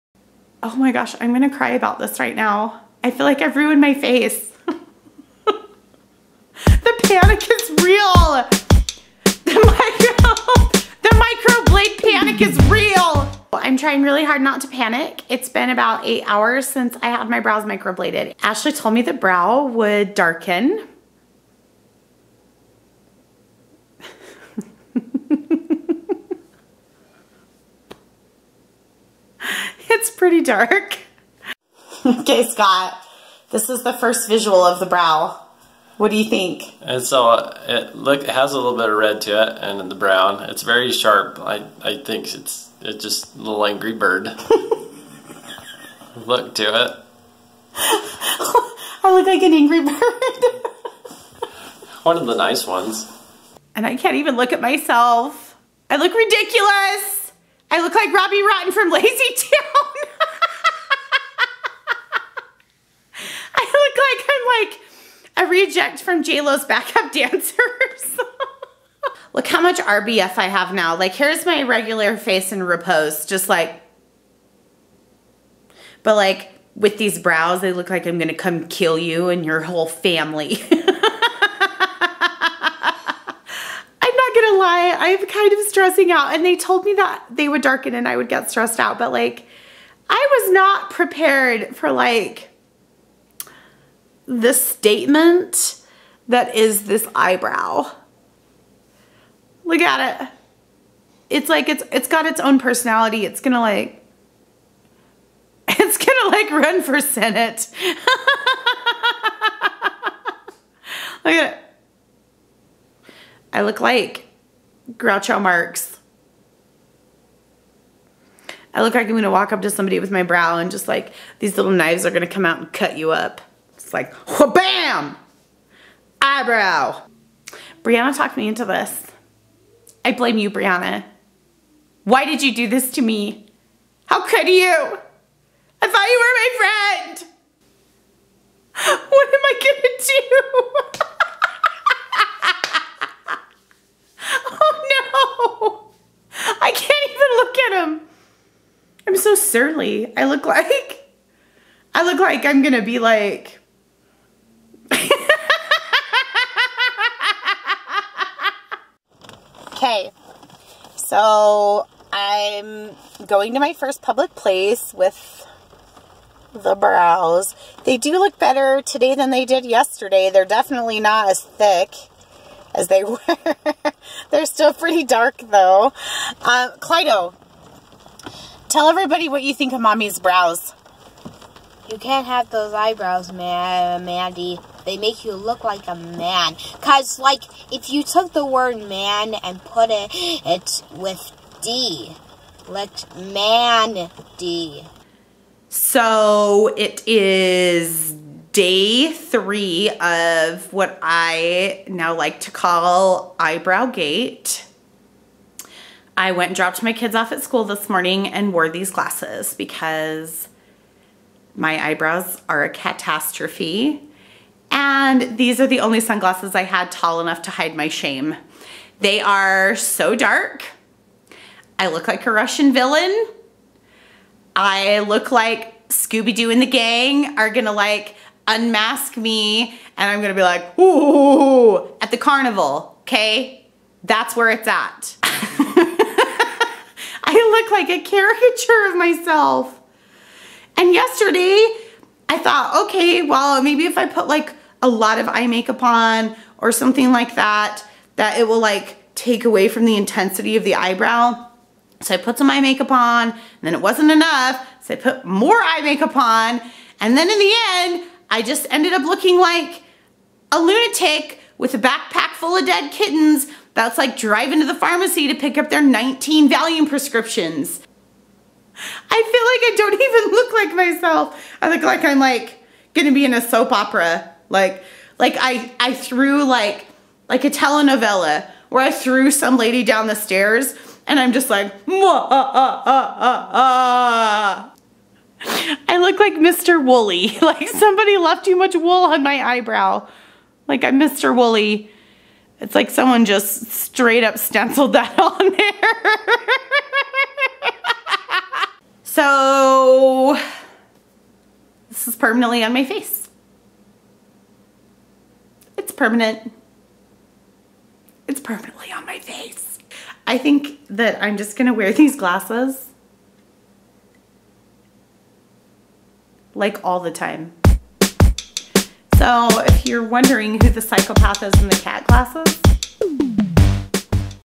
Mandy Microblades - The Panic Sets In
Everyone tries to warn you about the Microblade Panic you experience during the healing process, but it's impossible to be prepared. Watch me freak out about it for awhile.